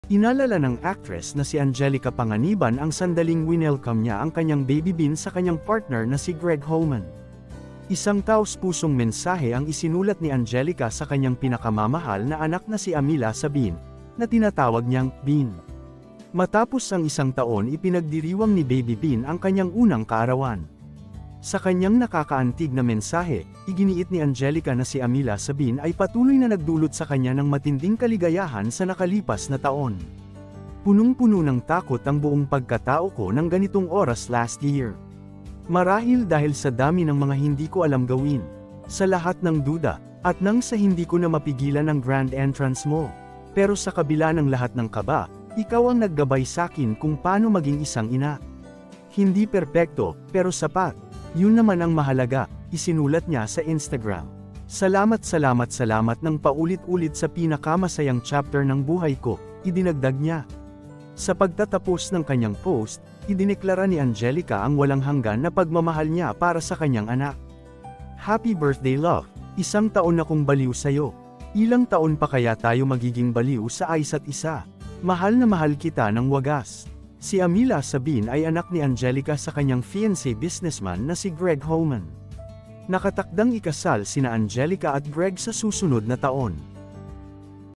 Angelica Panganiban Ipinagdiriwang ang unang kaarawan ng kanyang anak
Inalala ng actress na si Angelica Panganiban ang sandaling winelcome niya ang kanyang baby Bean sa kanyang partner na si Gregg Homan.